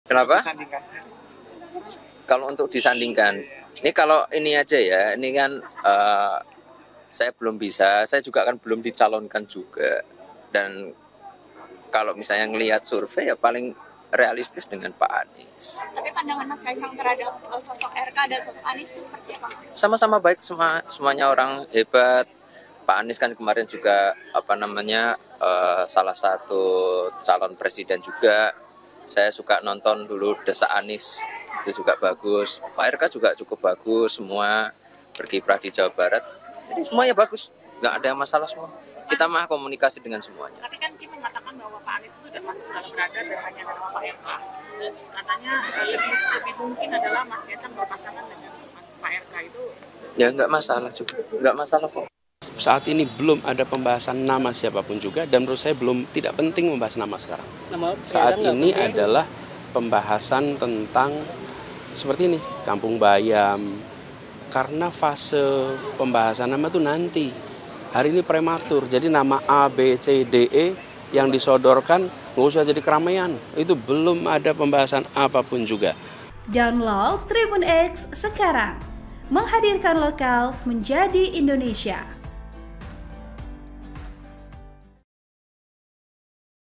MENOHOK! Kaesang Pengarep Soal Pilkada DKI, Sebut Paling Realistis dengan Anies Baswedan
TRIBUN-MEDAN.COM - Ketua Umum PSI Kaesang Pengarep mengatakan bahwa paling realistis untuk saat ini dirinya dipasangkan dengan Anies Baswedan untuk Pilkada DKI.

Kaesang menilai bahwa survei Anies paling tinggi sampai saat ini.

Kaesang tak mau banyak berkomentar, karena dia belum tentu maju dalam Pilkada tersebut.

Ia mengaku belum dicalonkan dari mana pun.

Sebelumnya, Anies Baswedan juga telah menerima dukungan dari DPW PKB Jakarta untuk menjadi Calon Tunggal Gubernur Jakarta pada Pilkada 2024.

Editor Video : Fanry Maulana